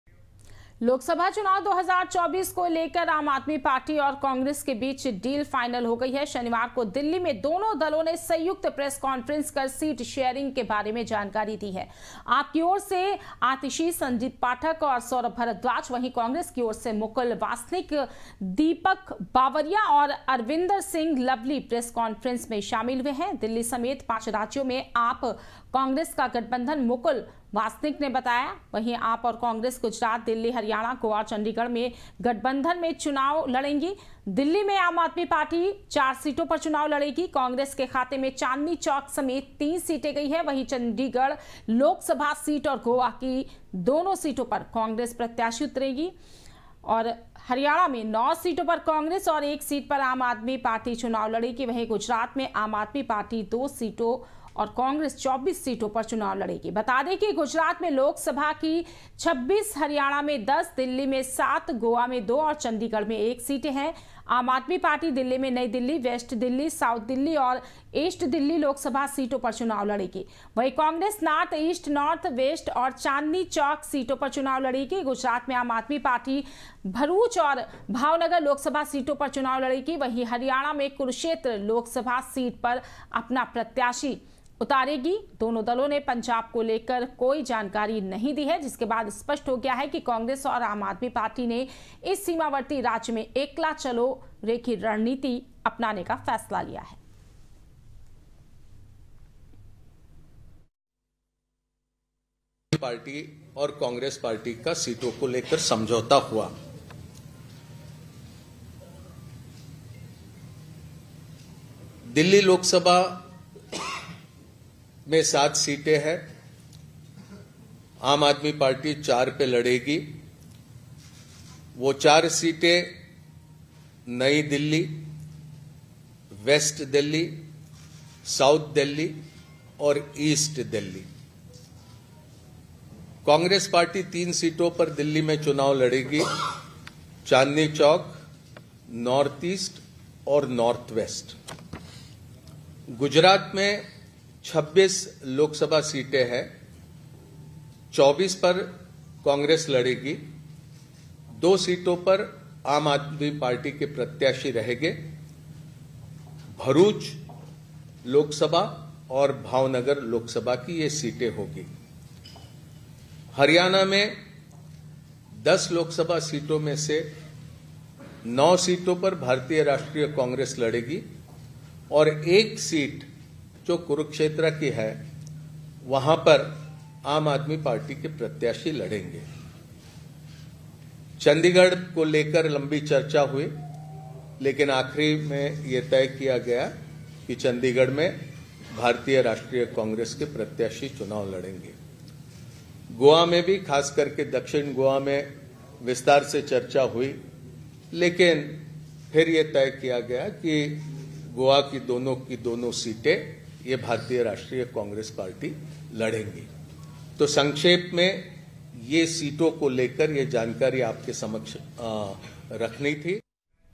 नई दिल्ली : AAP और कांग्रेस के बीच डील फाइनल, दोनों दलों ने की संयुक्त प्रेस कॉन्फ्रेंस | New Delhi
DEN- 157
SITI –85
JGD-99